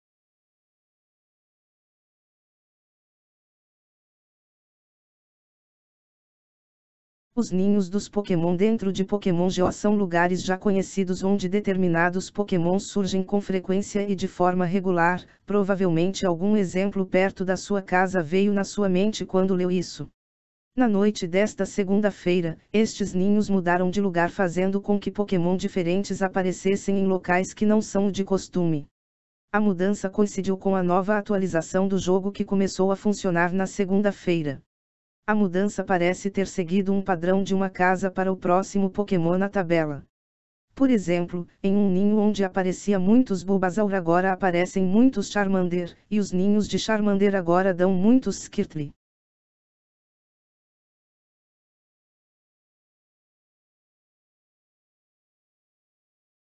Pokémon GO – “Ninhos” Dos Pokémon Mudaram De Lugar No Jogo!
Os “ninhos” dos Pokémon dentro de Pokémon GO são lugares já conhecidos onde determinados Pokémon surgem com frequência e de forma regular, provavelmente algum exemplo perto da sua casa veio na sua mente quando leu isso.